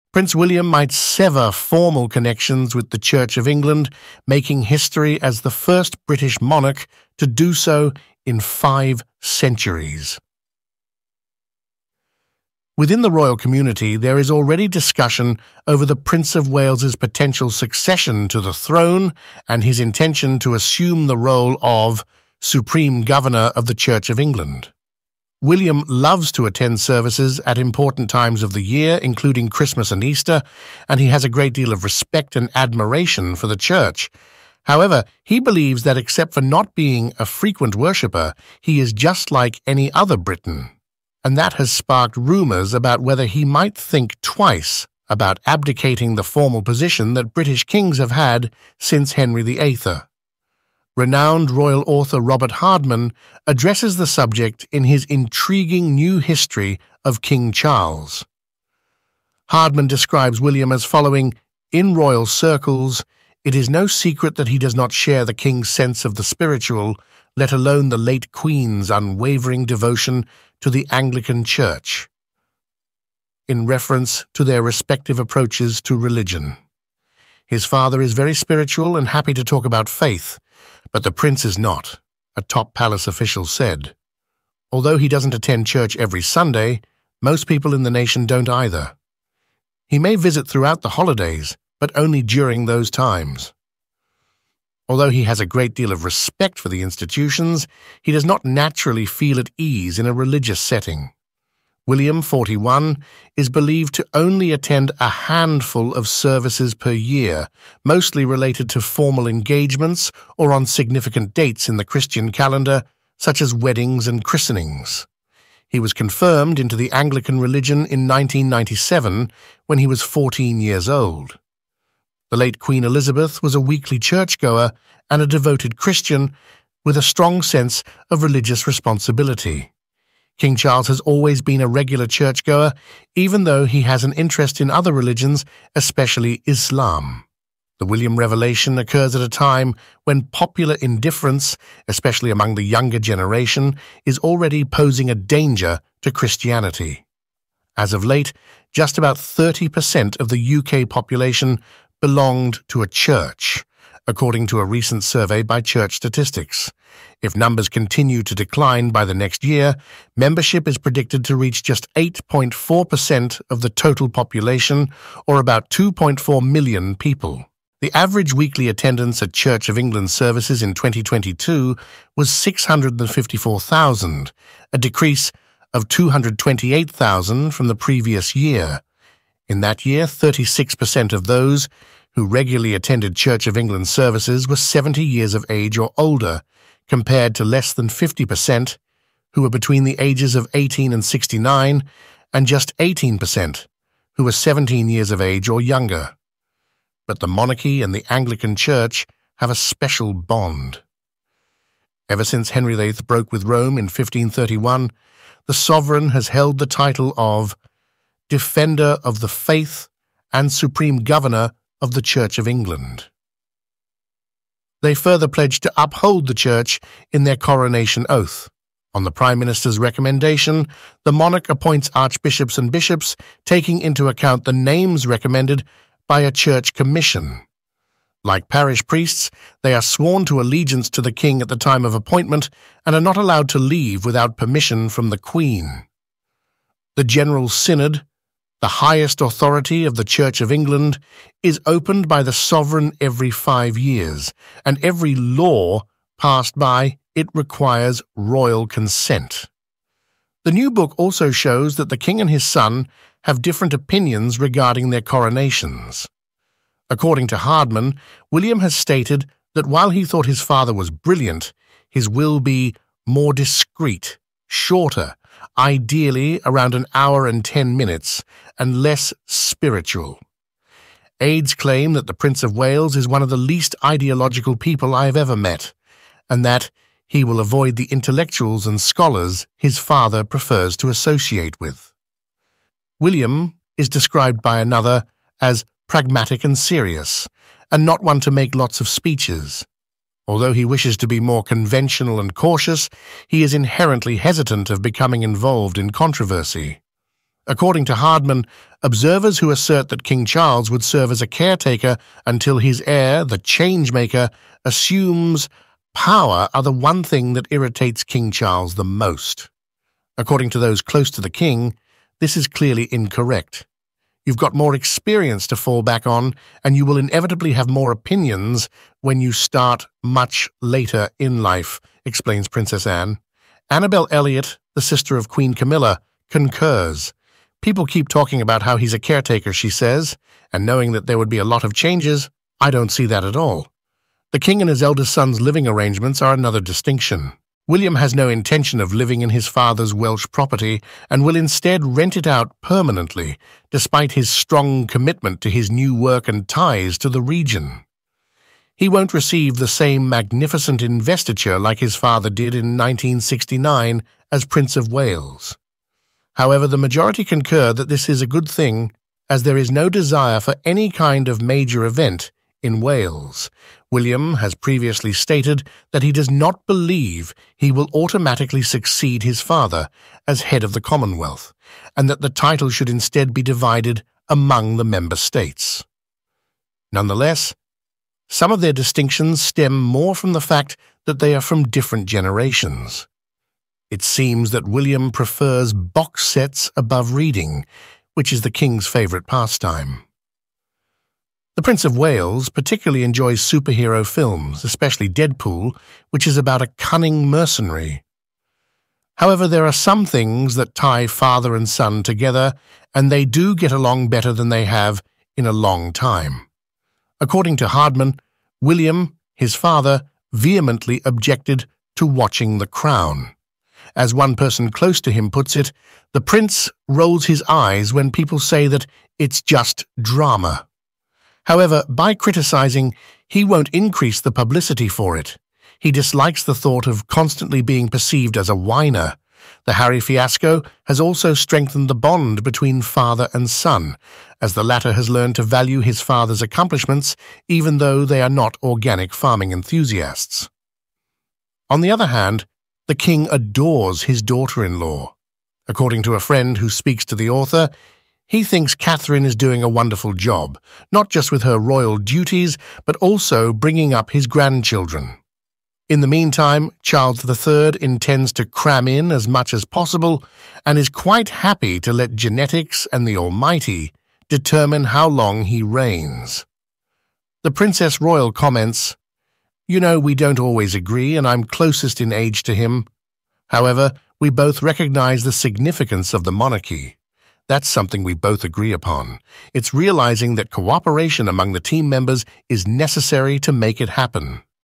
Prince William: A Secular King for a Less-Religious Britain?
Prince William, the second in line to the British throne, may become the first monarch in five centuries to not be the head of the Church of England. This is a controversial topic that has sparked debate among royal watchers and religious leaders. In this video, we will explore the reasons why Prince William may choose to break the tradition of his ancestors, and what implications this could have for the future of the monarchy and the church.

We will also look at the current state of Christianity in the UK, where church attendance and membership have been declining for decades, especially among the younger generation. How does Prince William’s personal faith, or lack thereof, reflect the changing attitudes of the British public towards religion? And how will this affect his relationship with the Anglican Church, which has a special bond with the monarchy?

We will also compare Prince William’s views and plans with those of his father, Prince Charles, who is expected to become the next king and the head of the church. How do they differ in their approach to the role of the monarch, the coronation ceremony, and the Commonwealth? And how will their decisions shape the future of the royal family and the nation?

Join us as we delve into the fascinating and complex topic of Prince William’s potential secular kingship, and what it means for the Church of England and the British society.